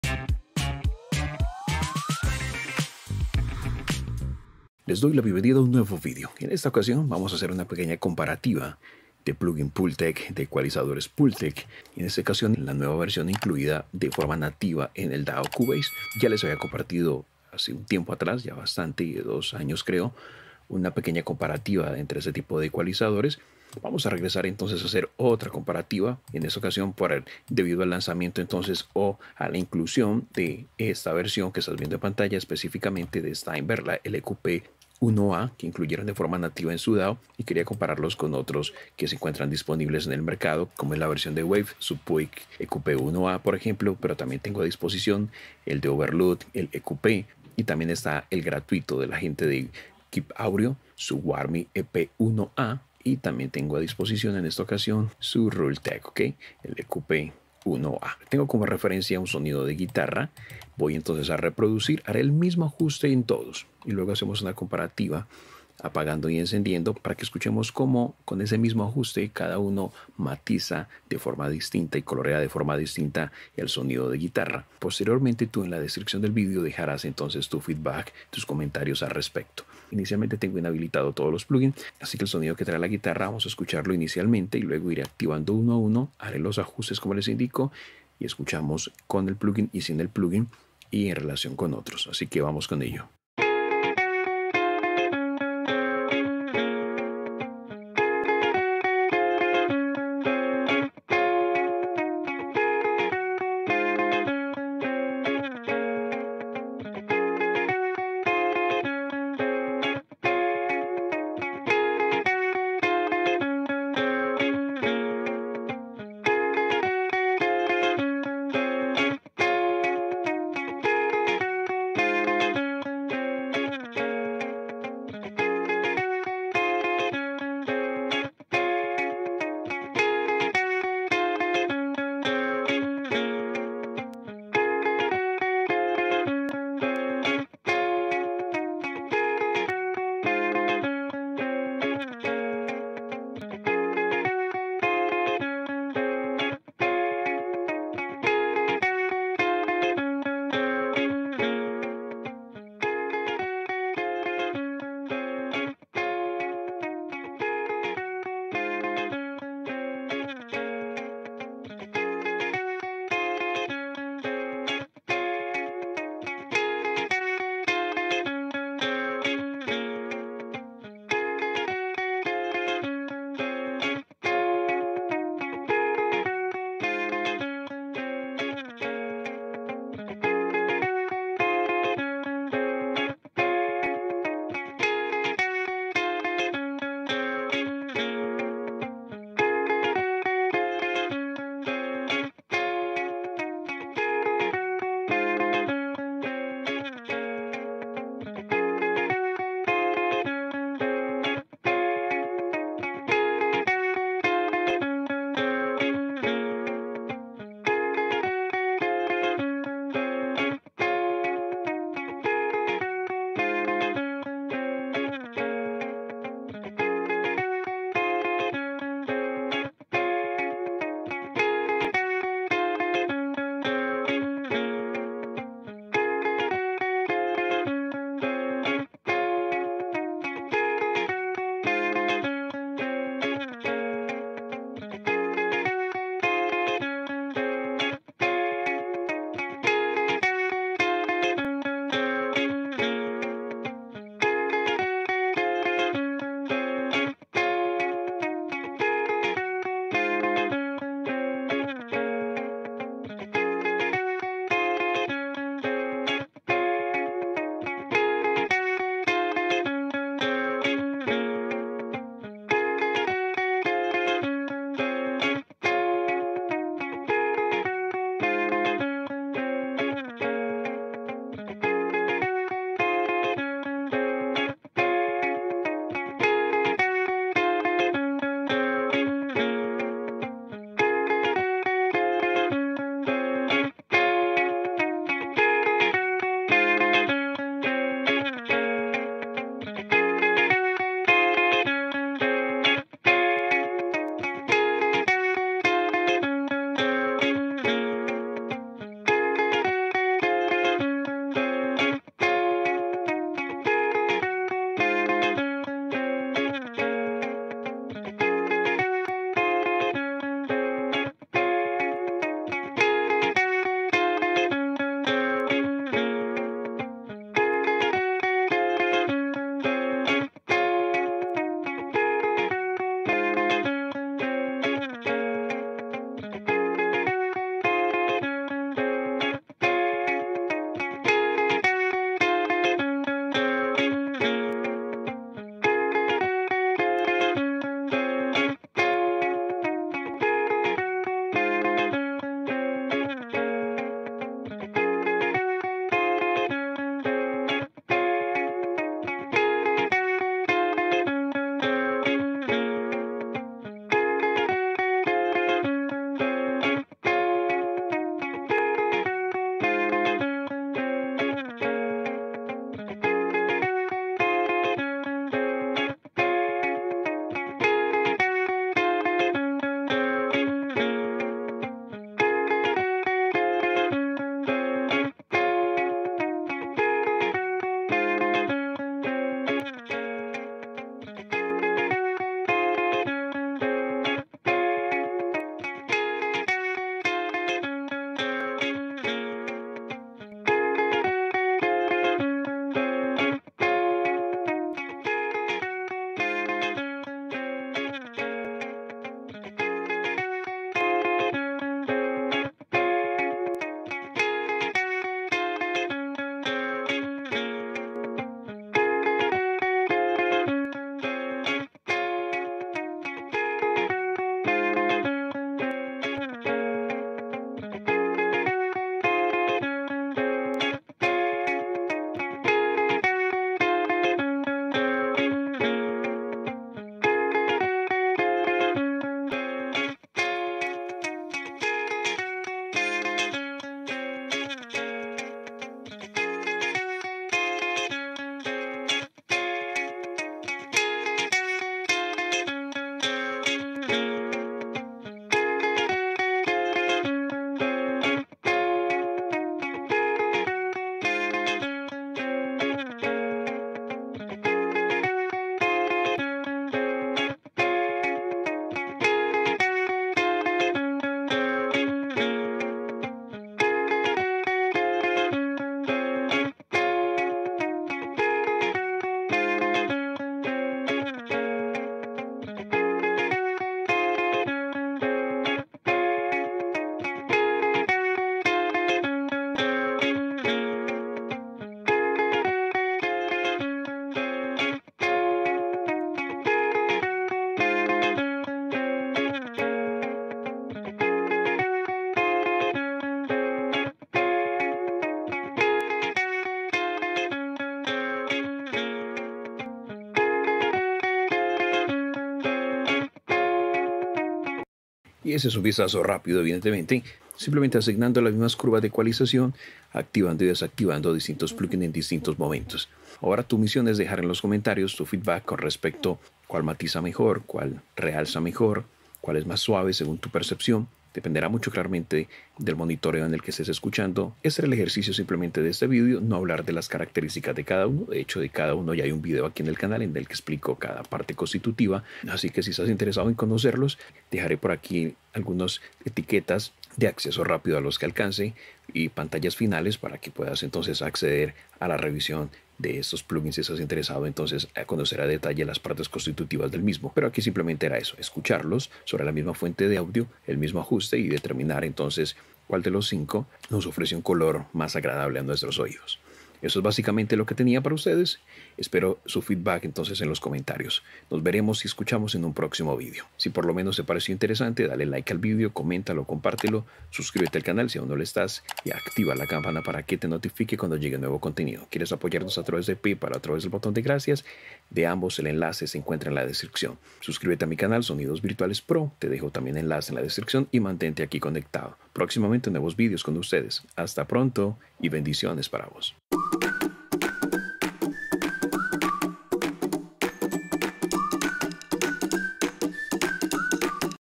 🎧Comparativa entre EQ´s Pultec🎧
Una comparativa de ecualización entre distintos modelos de EQ´s Pultec